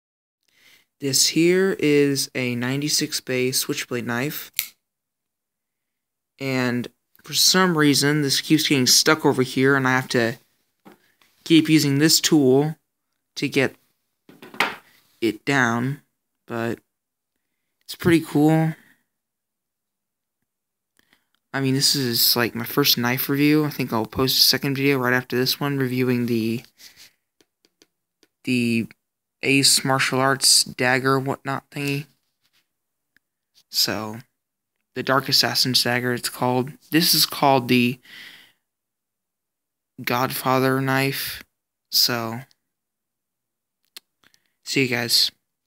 96 BAY CHROME BLACK HANDLE KNIFE REVIEW
In this video I show off my new knife